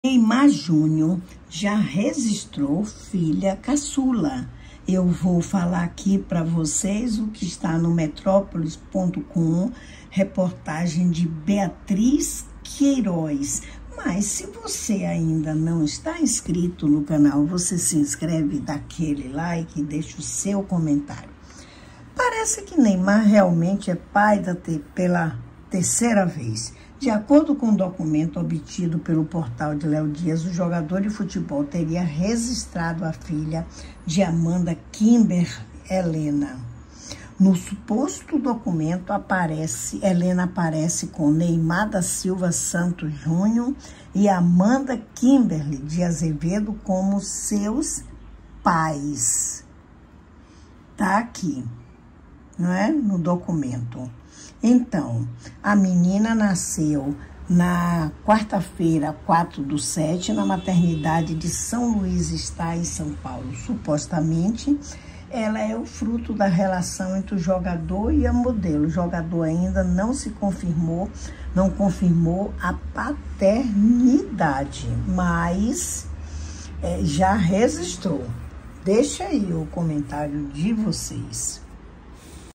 NEYMAR JR. JÁ REGISTROU SUA FILHA CAÇULA
Disseram que iria fazer DNA